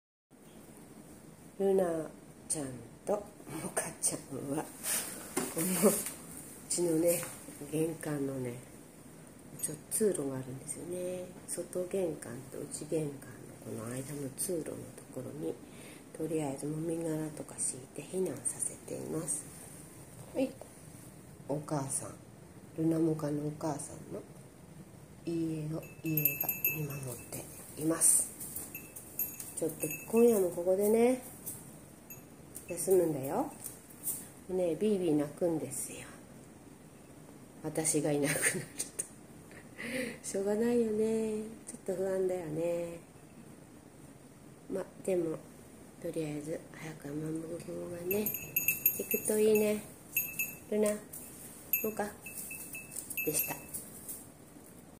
8/14子ヤギ2匹玄関脇通路に避難
豪雨によるヤギ小屋浸水のため避難